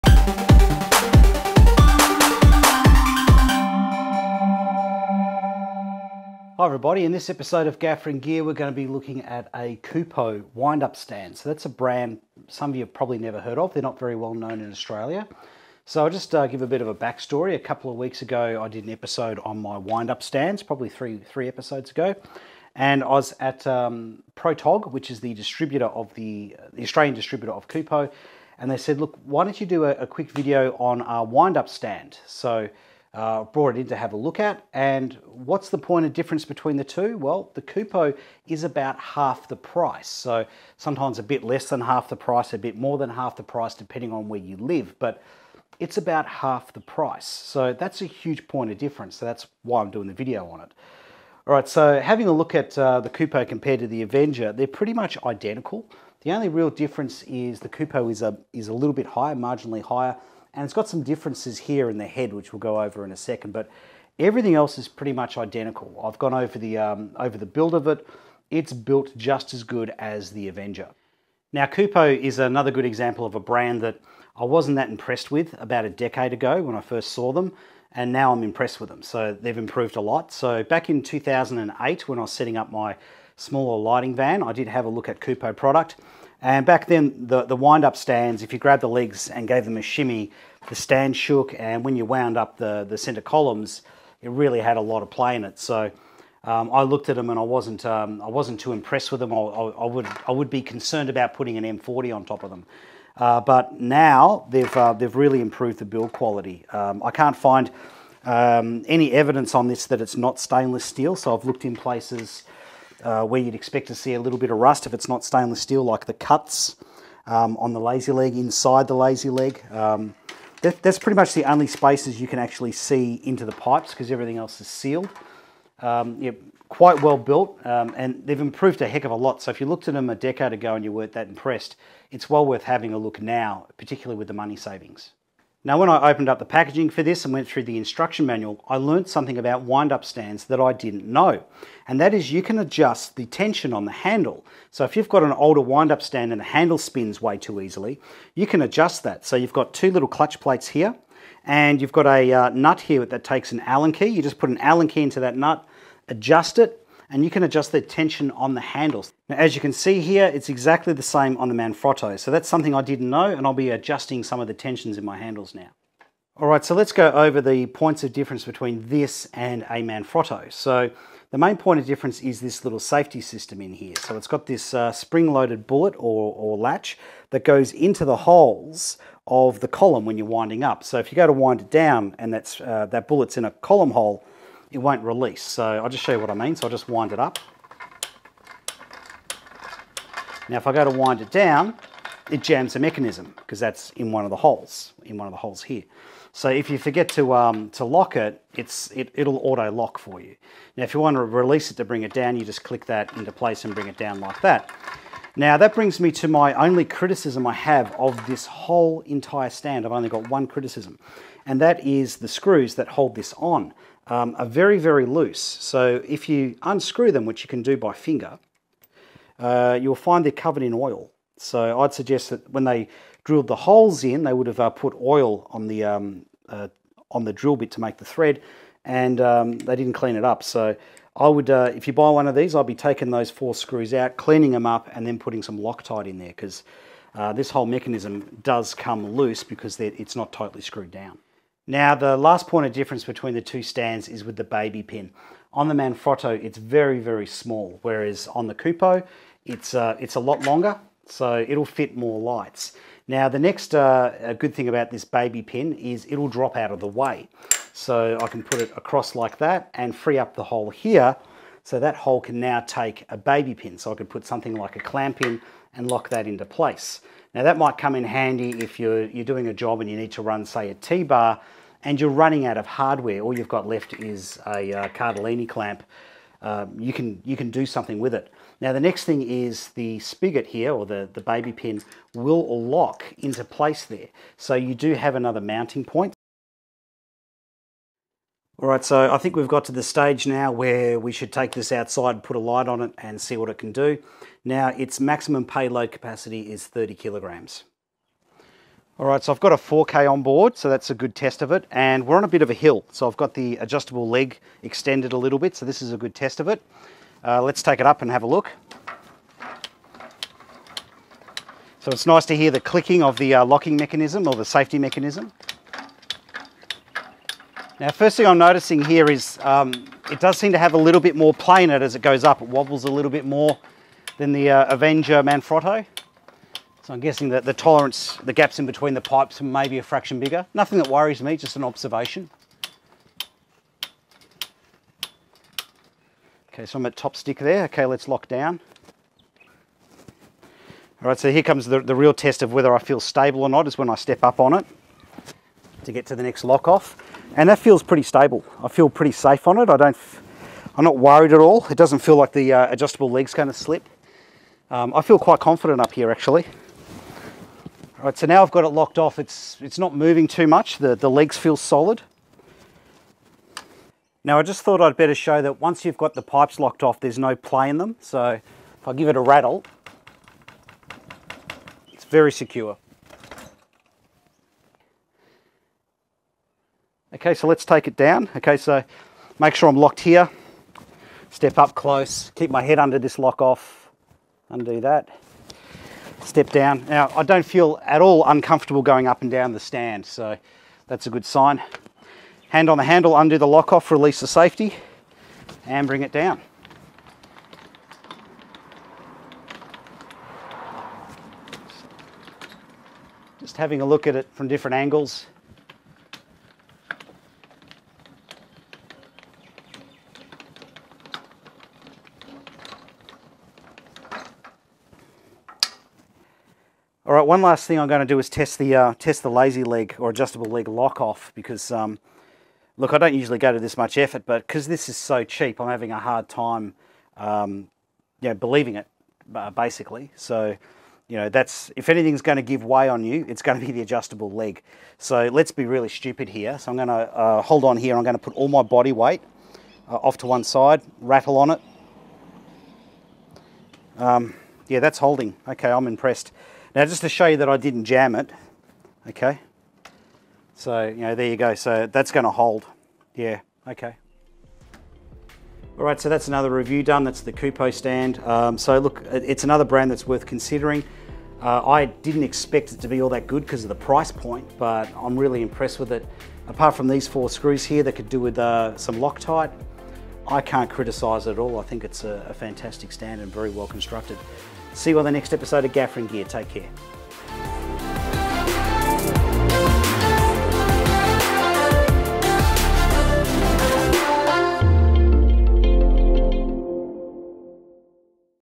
Gaffer & Gear 117 - Kupo Wind Up Stand
Taking a look at the Kupo 3 section wind-up stand (483T) and comparing it to the avenger.